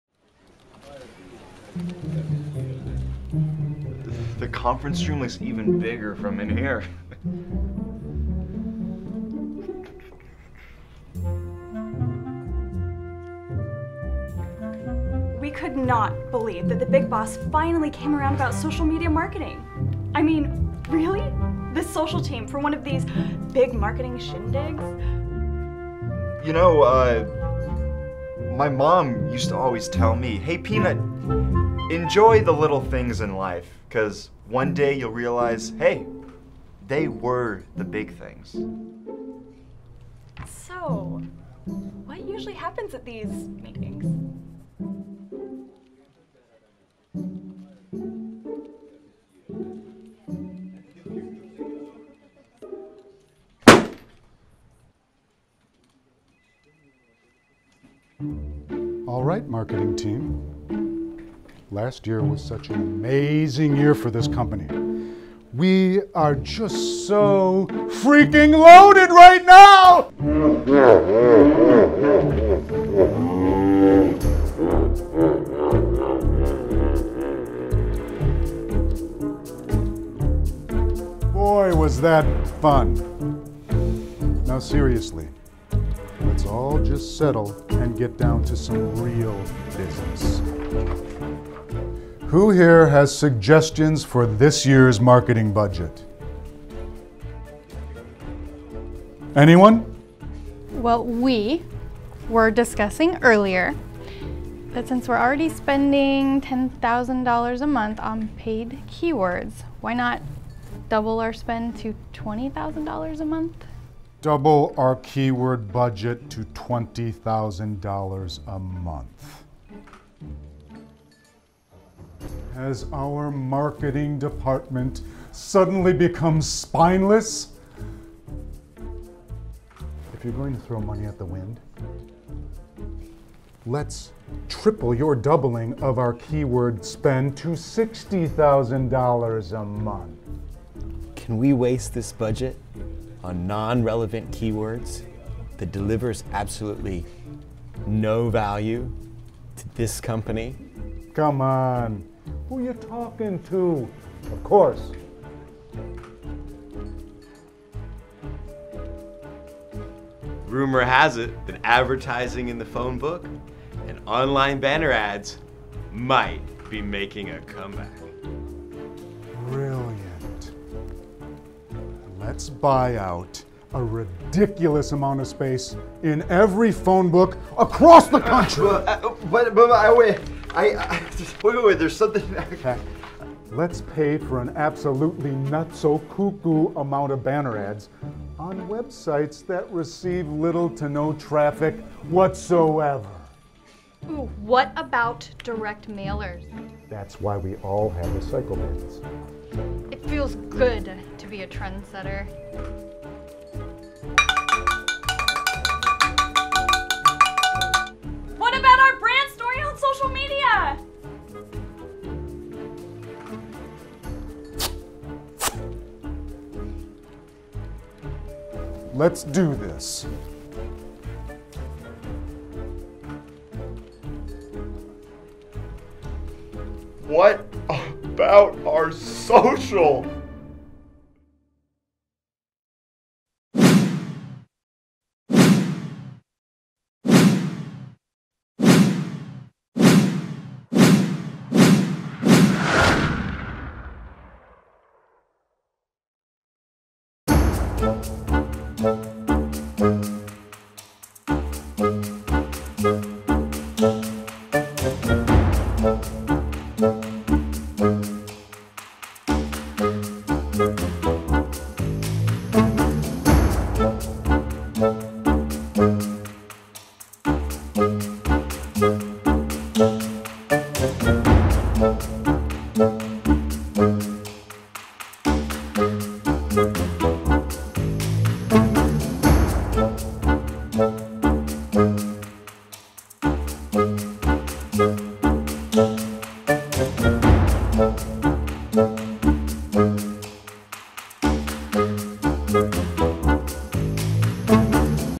Neglecting Your Brand Story on Social Media? Maybe It's Time to Show Social the Money!
See what happens when a social media marketing team is finally invited to join a real marketing meeting at this company.